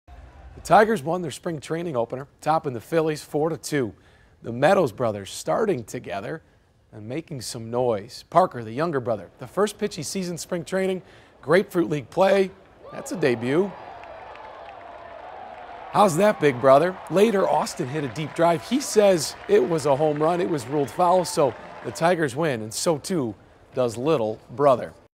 Parker Meadows homers in Tigers spring opener
Parker Meadows homers in Tigers spring opener | Brad Galli has more